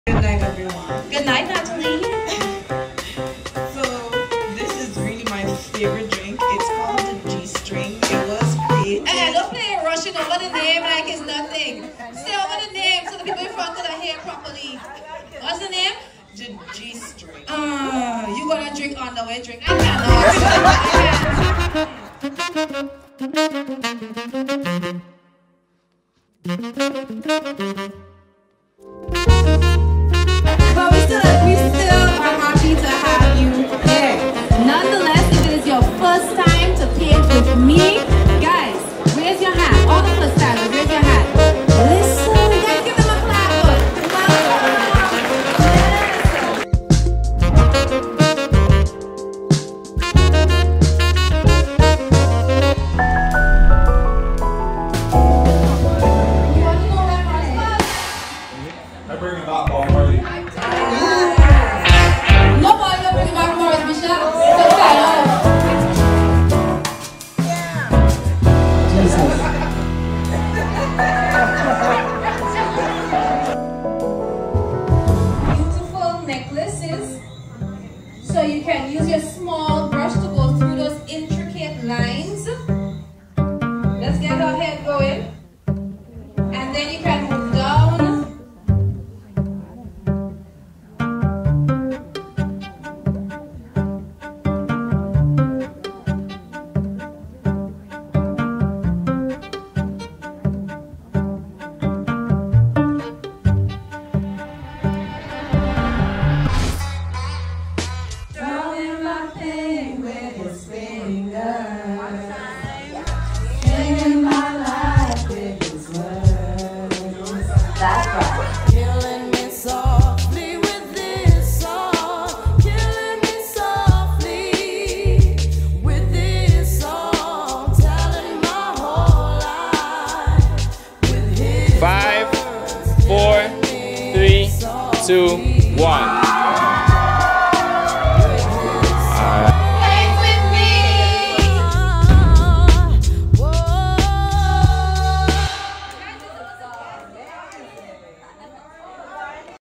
An unforgettable 100+ Painting Session.
A Paint, Sip and Relaxing moment With Friends and Family. Join the fun, lets see how much you may have missed!